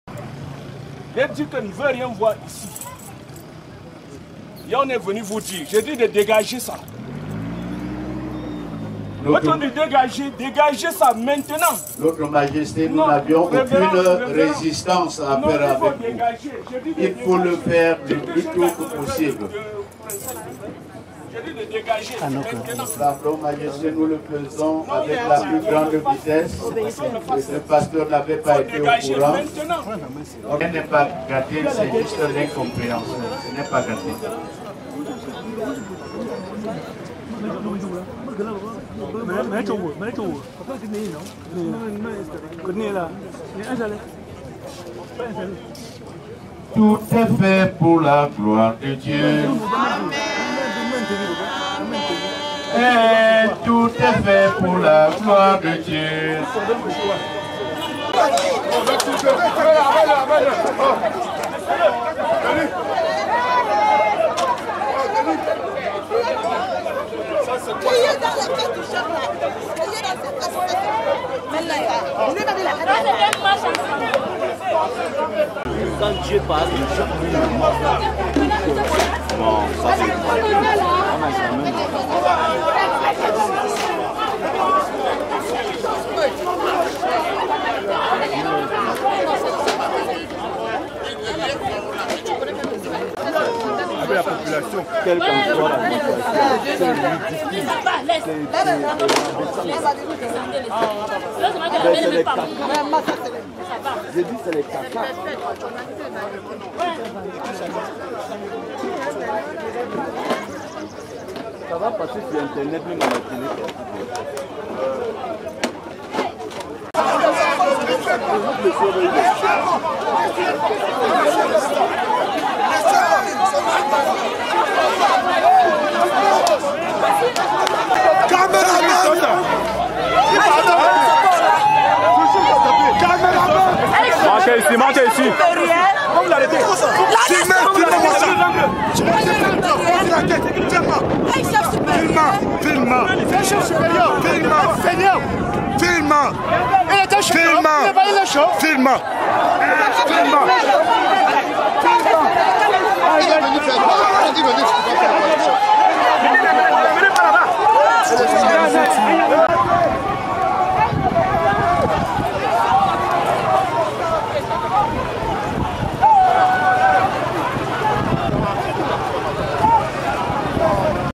Bangoulap. Quel honte 😤😠🤯 au Chef Bangoulap, a qui la Royauté monte au cerveau.
Je condamne cet acte, il faut respecter les deniers hommage de qui que ce soit. Même le pasteur t'appelle, mais tu t'en fou parce que tu es roi. La violence n'a jamais résolu un problème, mais créé des tentions et le regret de certains actes. J'aime Bangoulap, mais pas les animateurs de violence. Un papa, un grand père et chef de famille, tu portes ta canne sur sa tête. tes mains son tâche de sang honte 😤  toi.